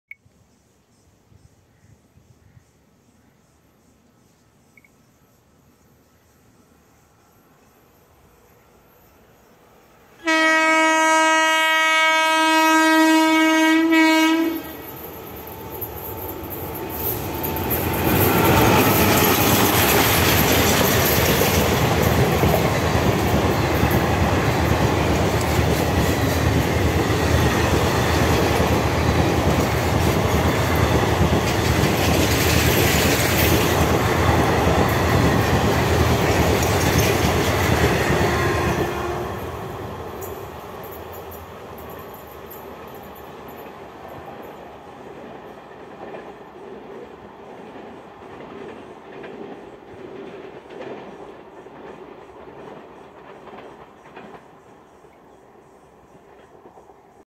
New Painted WAP4 AJJ Hauled Mangalore Coimbatore Intercity Superfast Express Skips Ullal.
Recording Date : 18 April 2022

22609 Mangaluru Central - Coimbatore Junction Intercity Superfast Express hauled with WAP4 Arakkonam skips Ullal railway station in Mangalore.